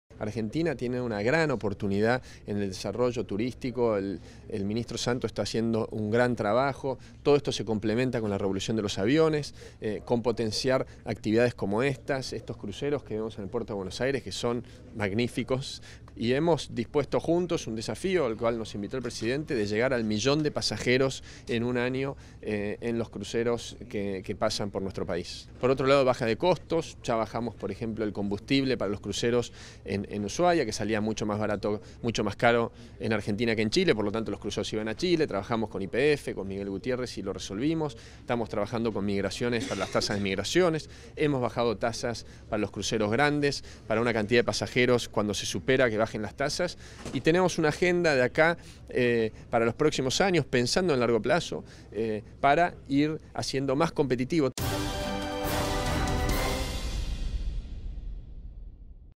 Dietrich espera que en un año lleguen al país un millón de turistas a bordo de cruceros
El ministro de Transporte de la Nación, Guillermo Dietrich, aseguró que espera que a lo largo del próximo año lleguen al país más de un millón de pasajeros a bordo de los cruceros turísticos, y para ello su cartera trabaja en la remodelación de los puertos de Buenos Aires, Ushuaia y Puerto Madryn.